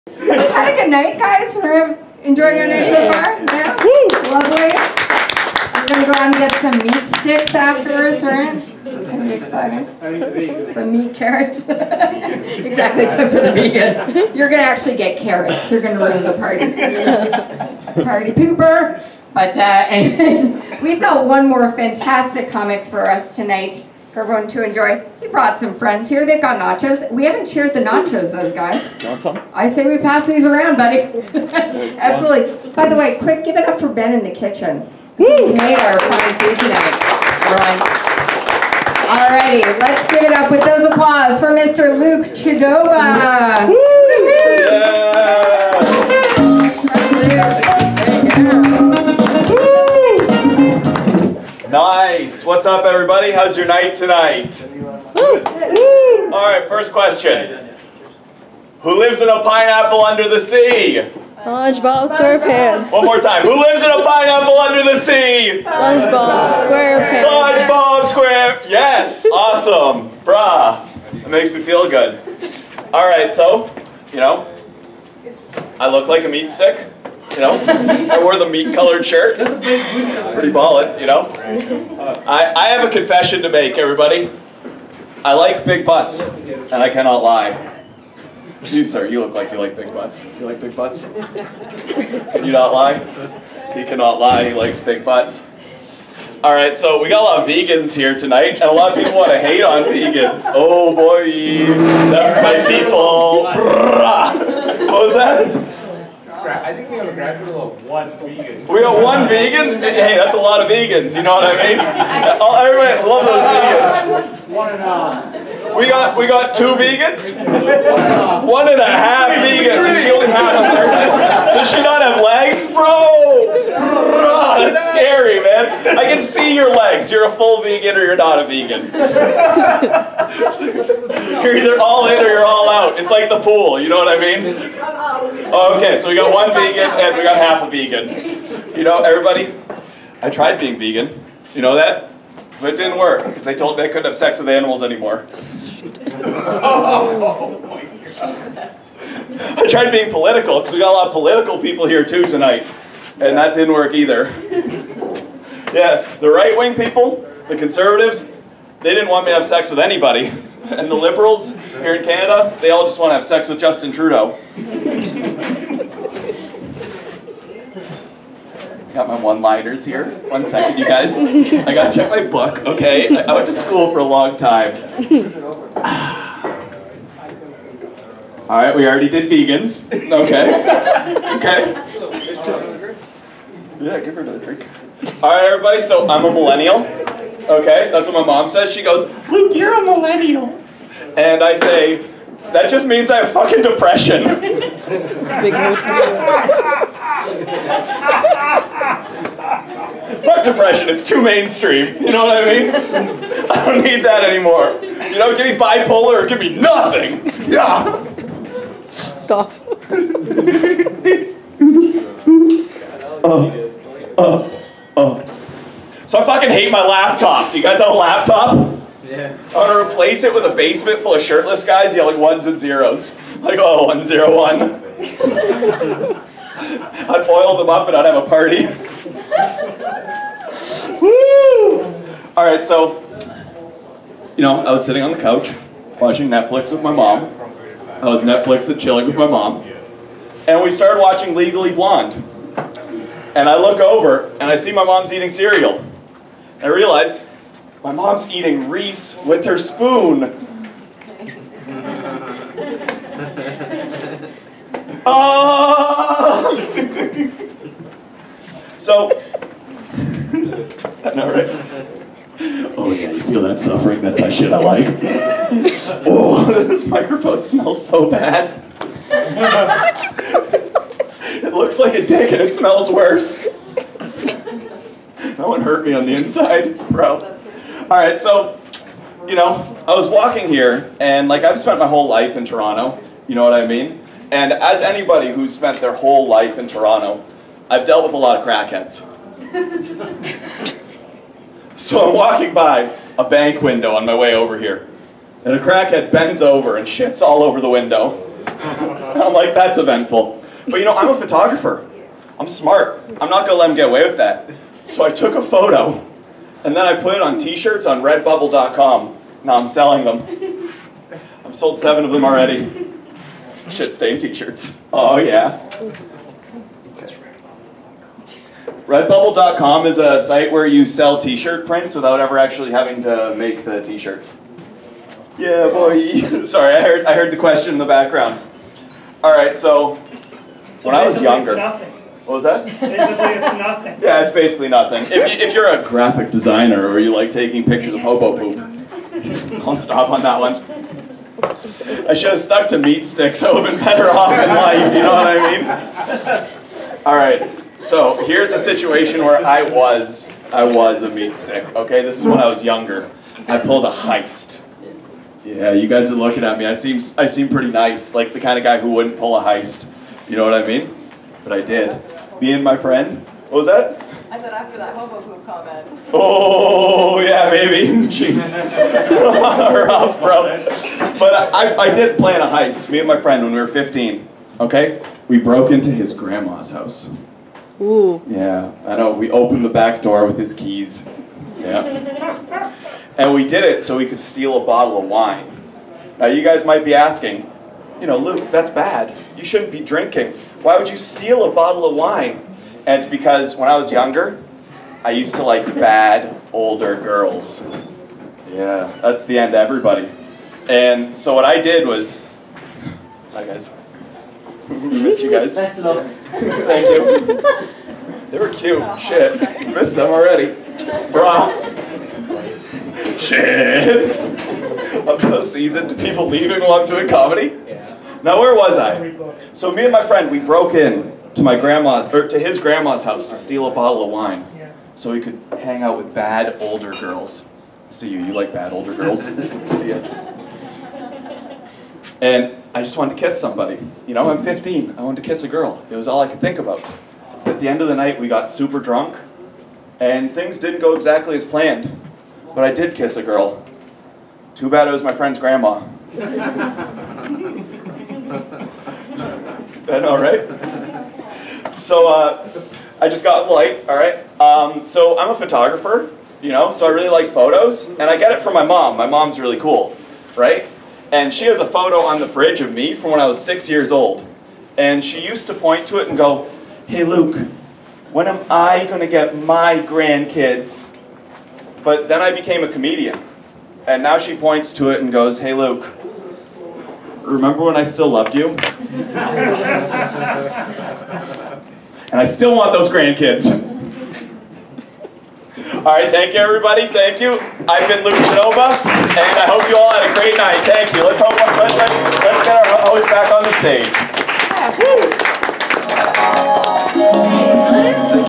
Luke Chudoba Stand Up Comedy at Einsteins Bierhalle 2019
I did a really good comedy show at Einstein's Bierhalle in Toronto. I had a great audience and I really enjoyed myself. This was one of my best shows yet and I really appreciate all the people who have been kind and supportive to me. Thank you.